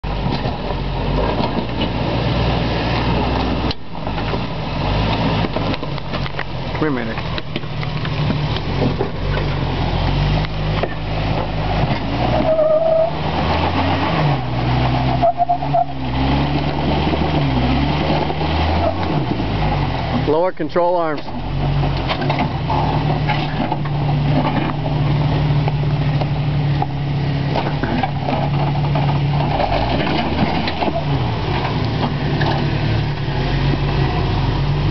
stock
Wheelin my TJ when it was stock.